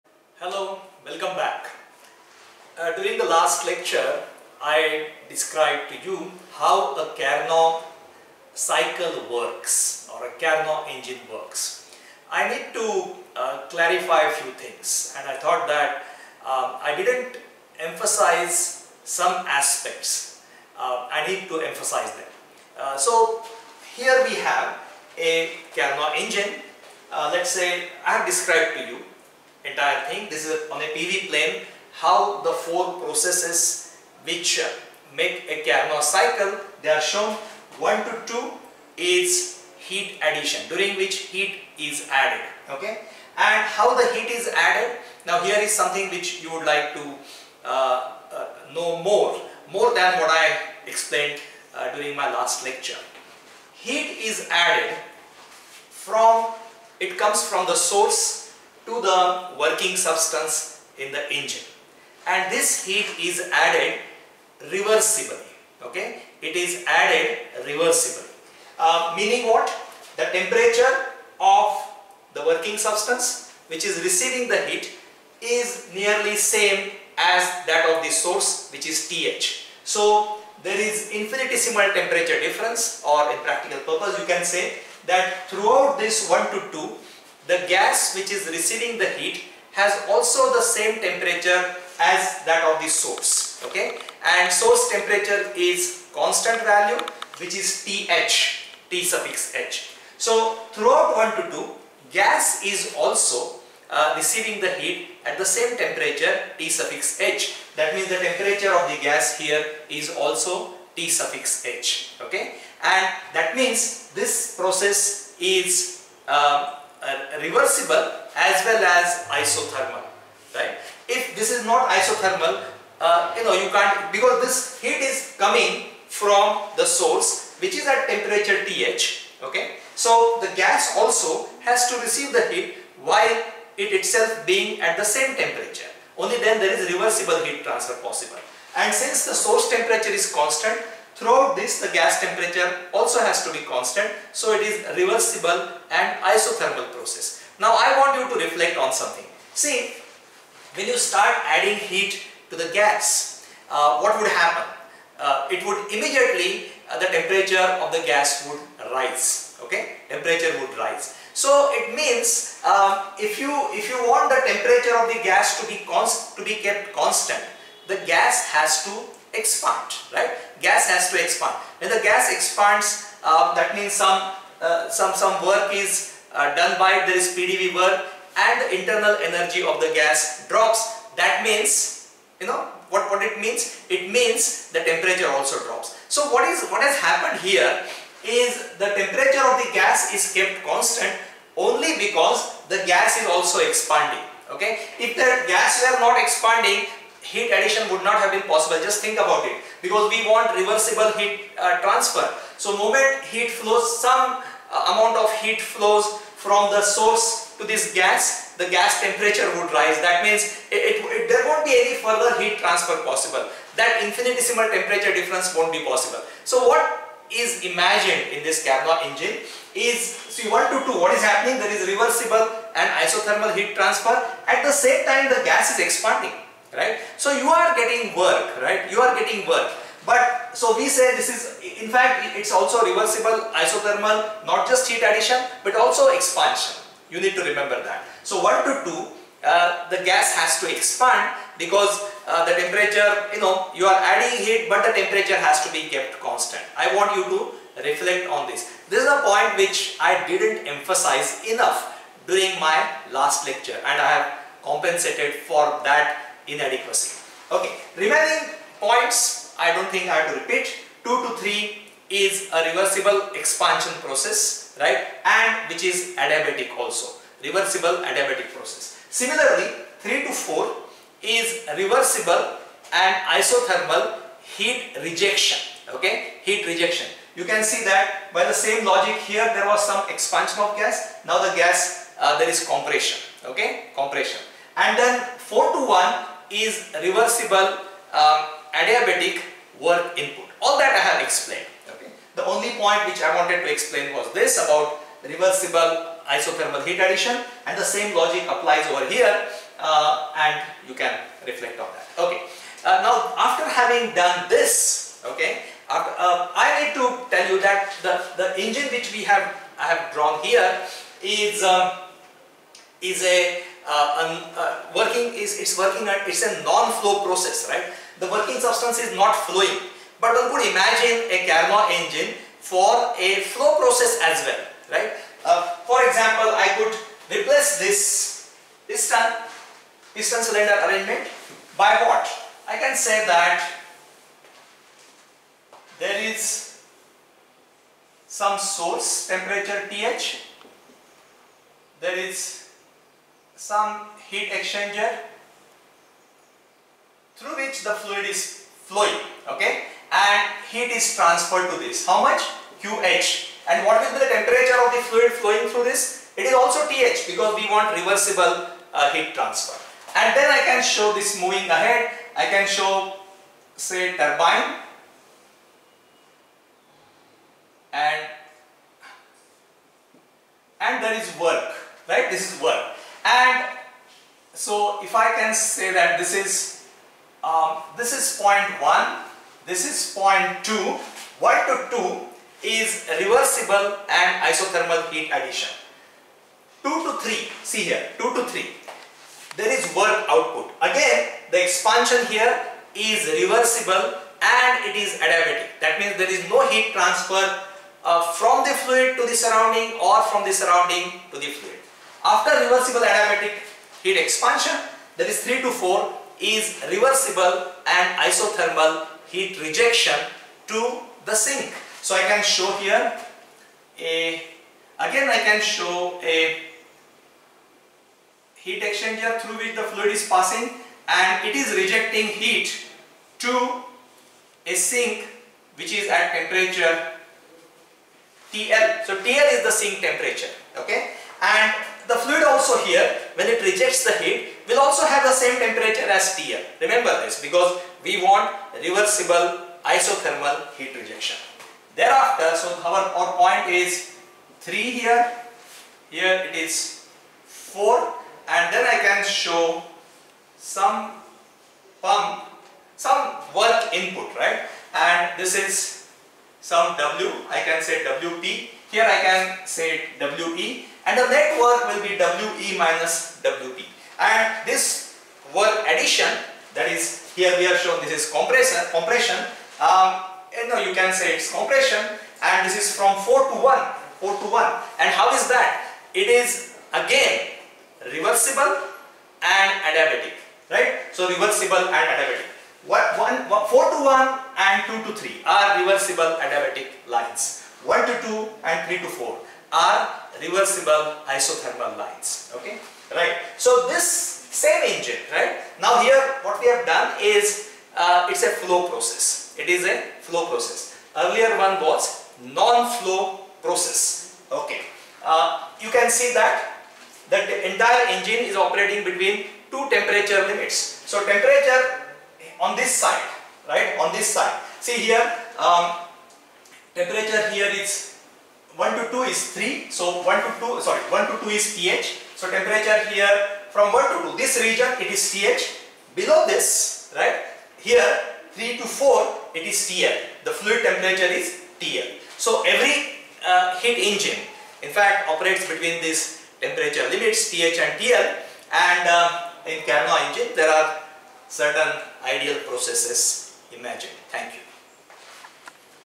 Video No.16:Additional explanation of the working of Carnot Engine
some features of heat addition ad heat rejection process